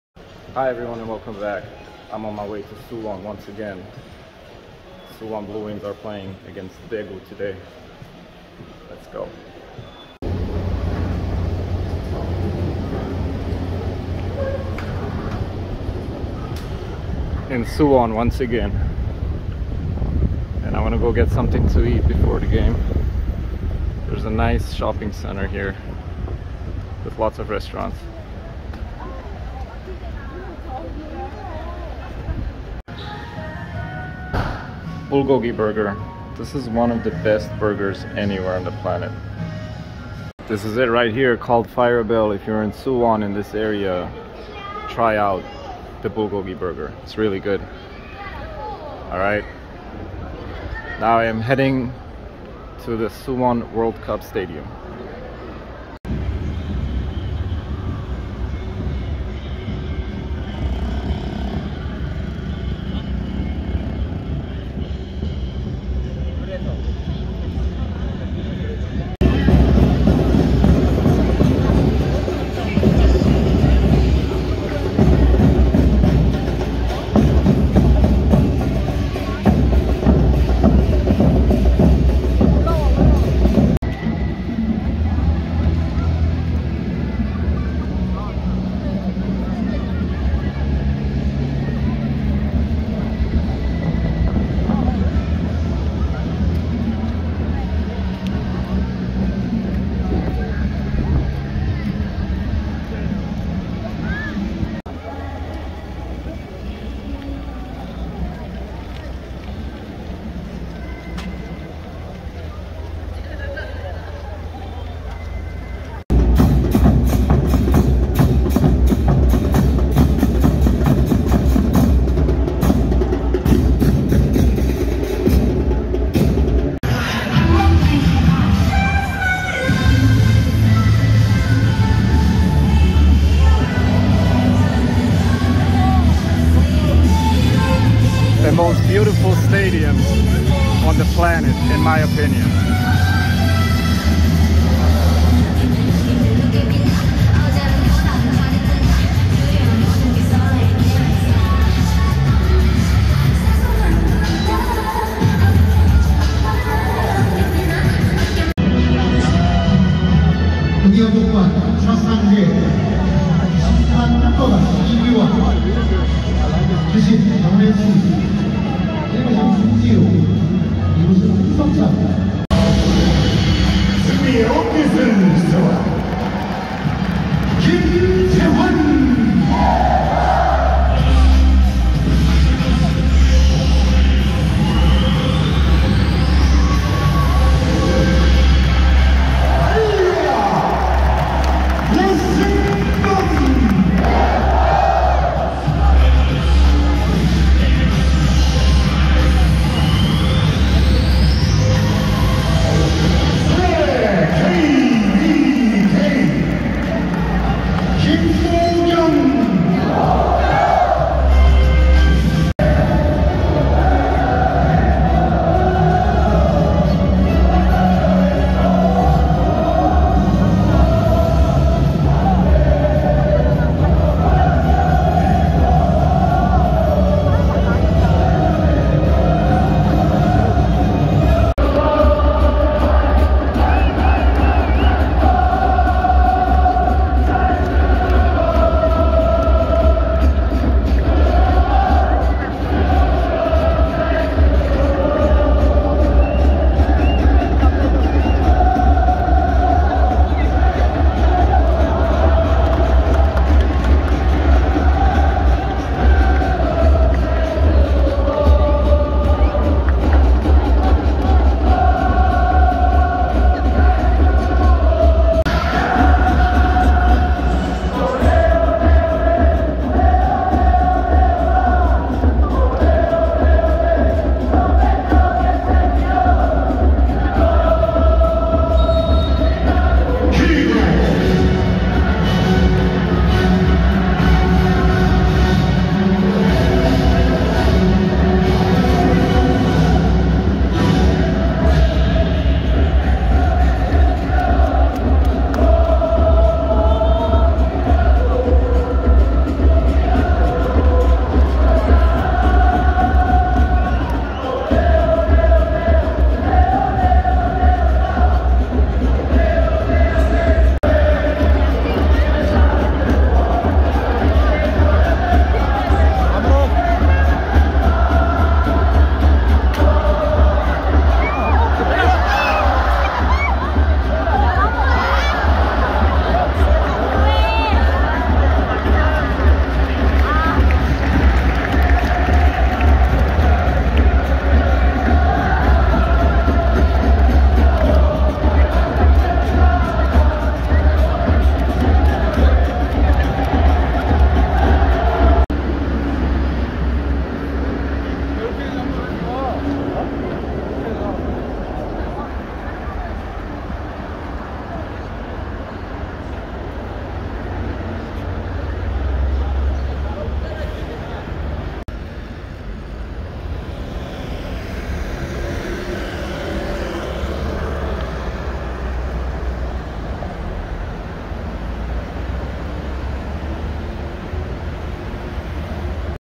Suwon - Daegu FC. Korean football K League 1. 수원 삼성 블루윙즈 - 대구 FC. Korejska Liga. Walking in Gangnam.
Once again I was at the Suwo. World Cup Stadium for a K League 1 game between Suwon Samsung Bluewings and Daegu FC. Walking around Gangnam at night. K 리그 1 수원 삼성 블루윙즈 - 대구 FC. 수원 월드컵경기장. Prva Korejska Liga. Utakmica između Suvon i Degu. Bulgogi Burger at Firebell.